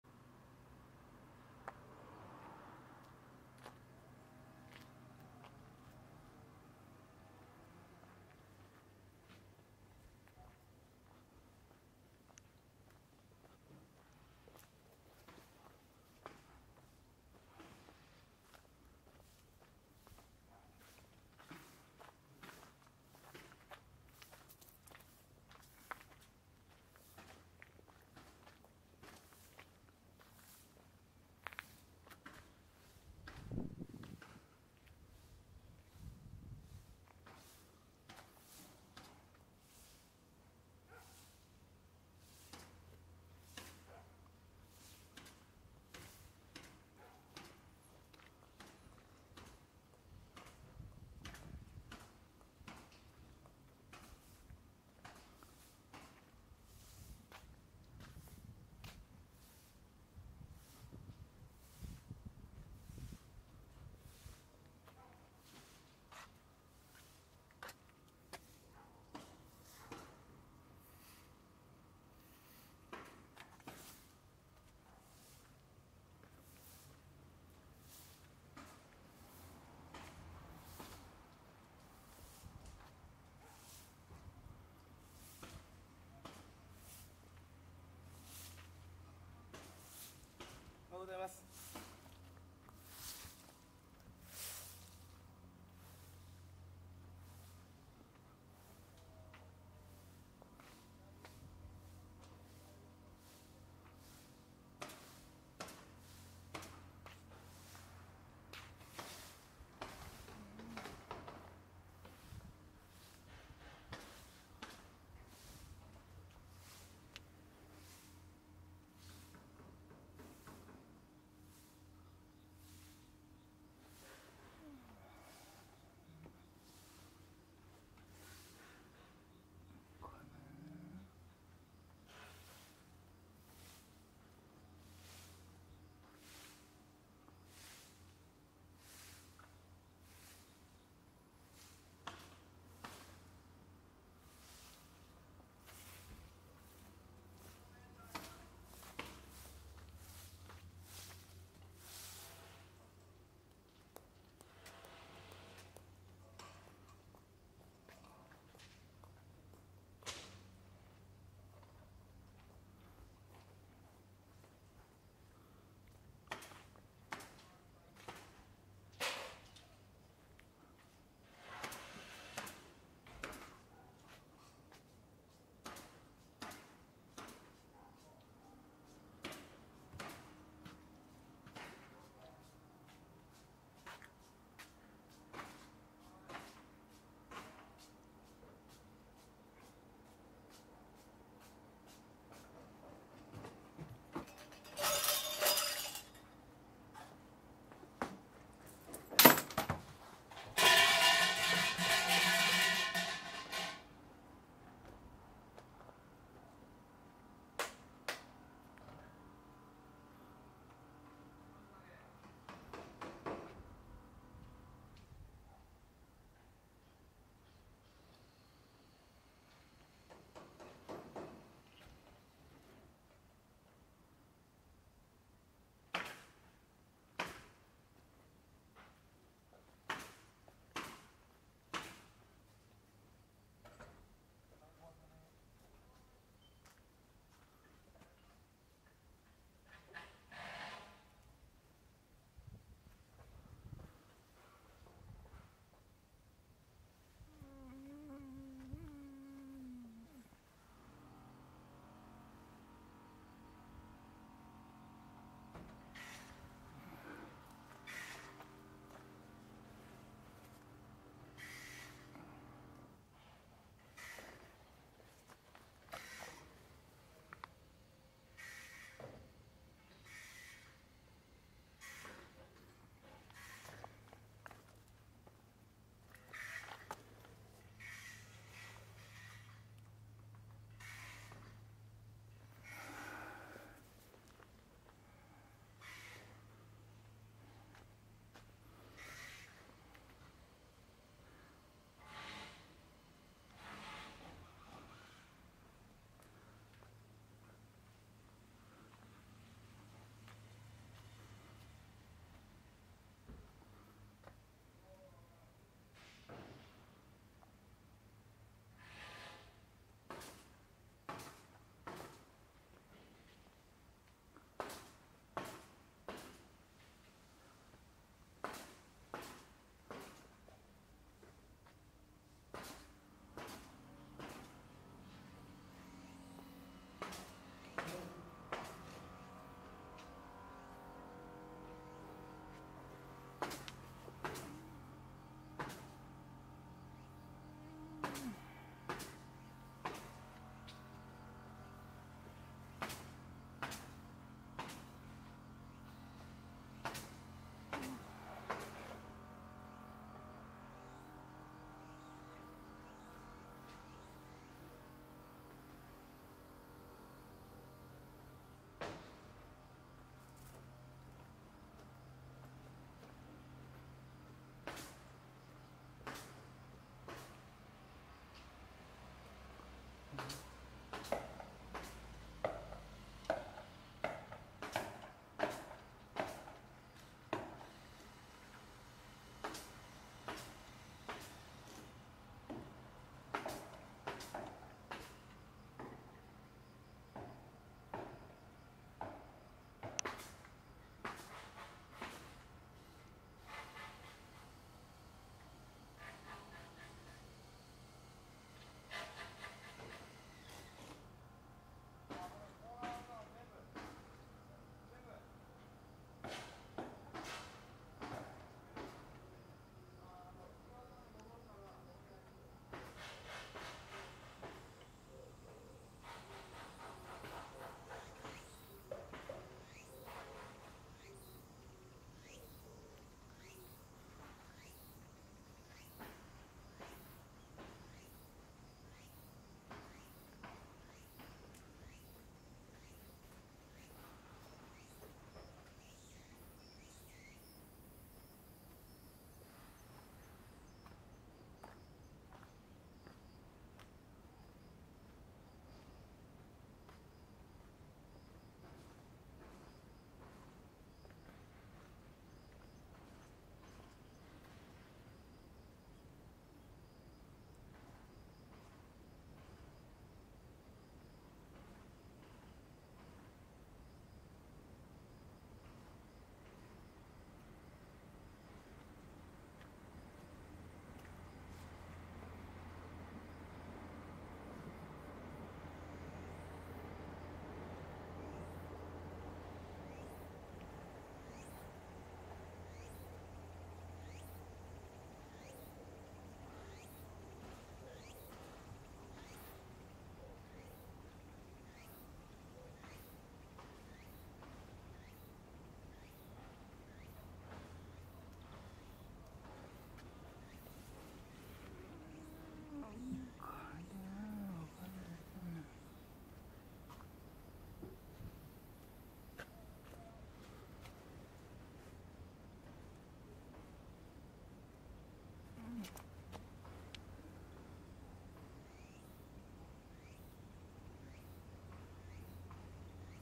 SJ8 PRO 4K30f V1.2.7 EIS 6軸補正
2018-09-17
15日に公開された新ヴァージョンでの4K30f撮影。
古河市中田1337、鶴峯八幡宮於
今回のヴァージョンは、play memoryでサムネール表示もされて、パソコンのスペックが良いなら、再生もスムーズに可能ですが、MP4変換は出来ないので、ファイル接続と静止画の切り出しは、出来ません。
同じ1080 60fでもSJ6とSJ8 PROのファイル形式は、違うらしい？
自社製品用の動画ソフトをSjcamが発表して呉れたら、解決ですが。